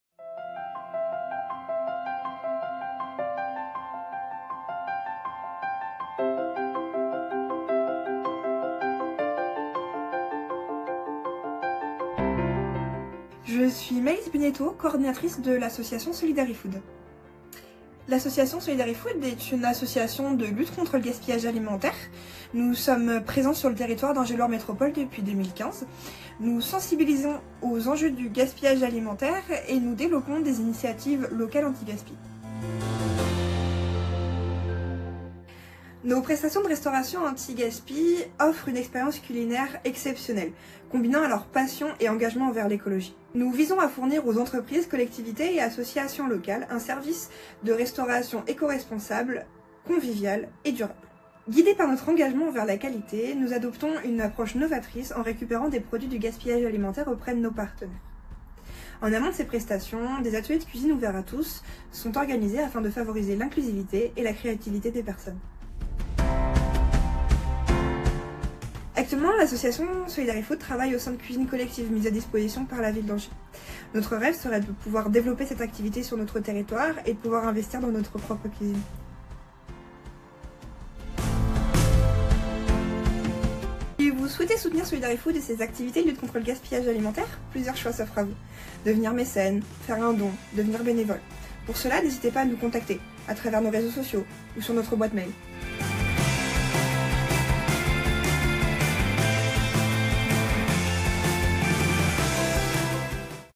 Solidarifood : Sa lutte contre le gaspillage alimentaire - Lauréat inspiré 2024
Dans cette vidéo, Maëlis Bénéteau, coordinatrice de Solidarifood, basée à Angers depuis 2015, nous explique comment l'association sensibilise aux enjeux du gaspillage alimentaire et développe des initiatives locales pour y remédier. Solidarifood adopte une approche innovante en récupérant des produits alimentaires auprès de ses partenaires pour ses prestations. Des ateliers de cuisine ouverts à tou.te.s sont également organisés pour encourager l'inclusivité et la créativité. Actuellement basée dans des cuisines collectives mises à disposition par la ville d'Angers, Solidarifood aspire à développer son activité et investir dans sa propre cuisine. Soutenez l'association en devenant mécène, en faisant un don ou en devenant bénévole.

0:00 Introduction
0:13 Présentation Solidarifood
0:34 Que fait Solidarifood ?
1:07 Quel est le rêve de Solidarifood ?
1:22 Leur dernier mot